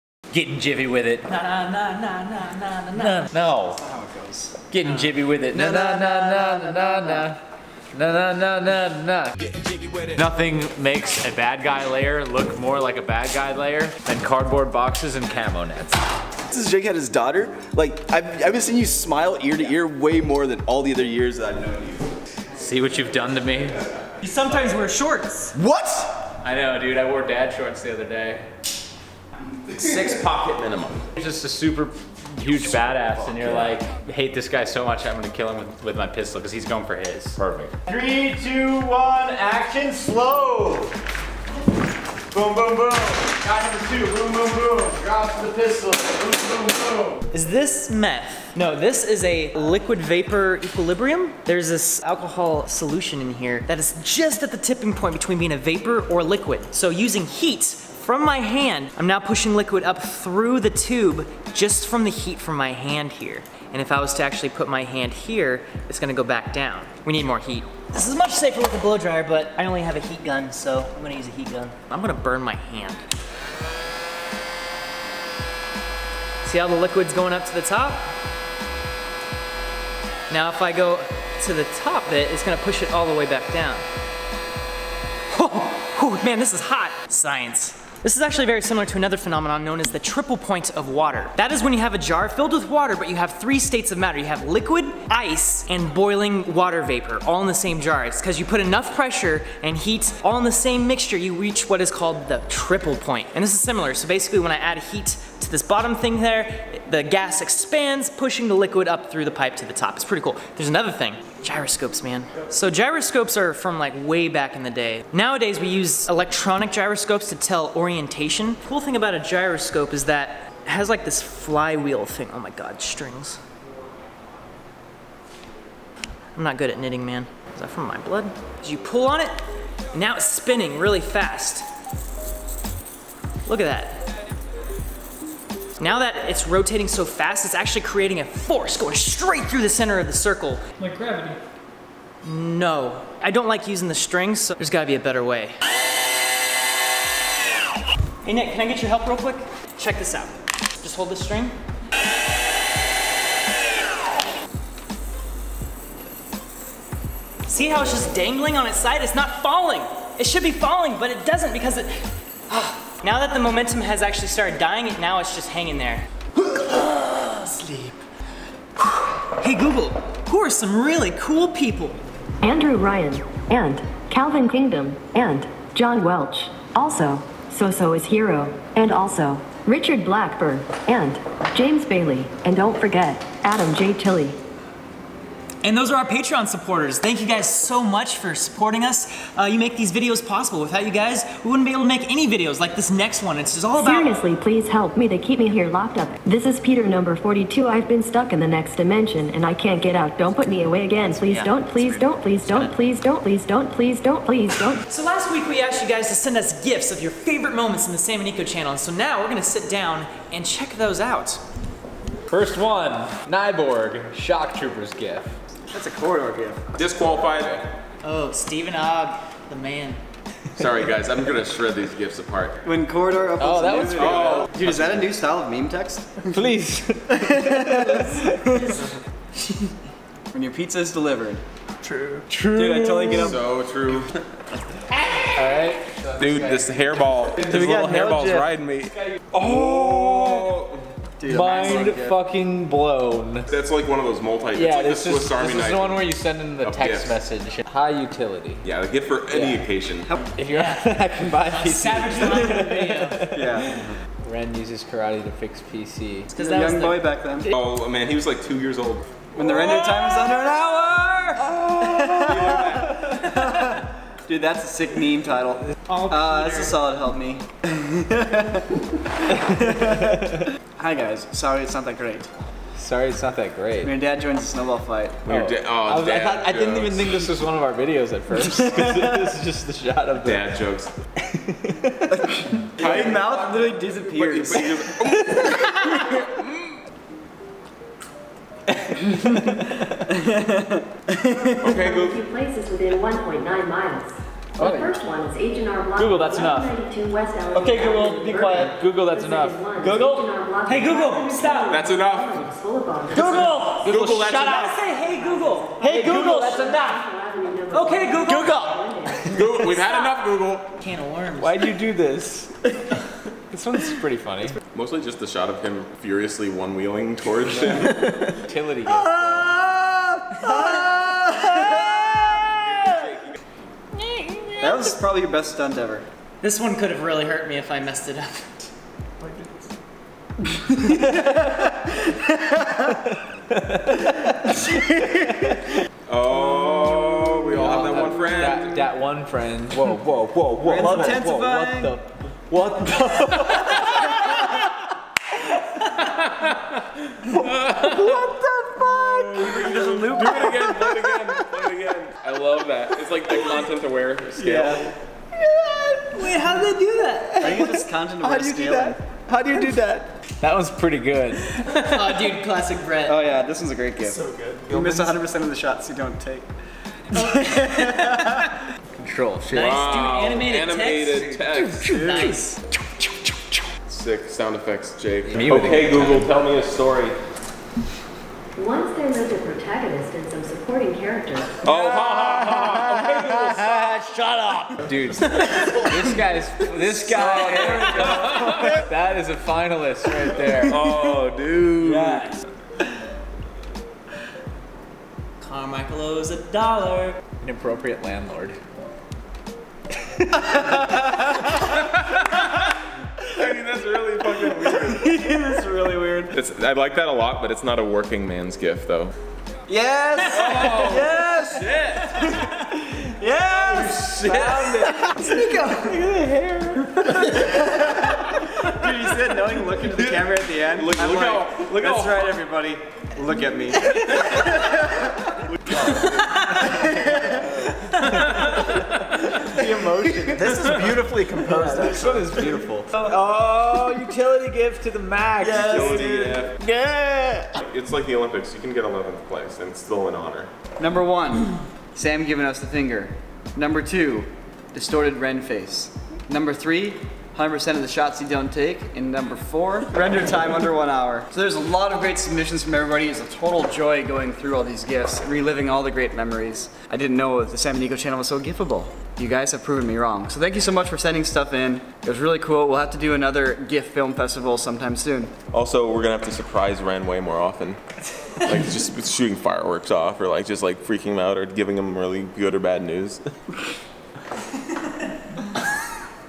Defying Gravity with Science!
Thanks for all the Gifs!!

This Episode ► Wren defies gravity with heat, and the crew judges the gif contest.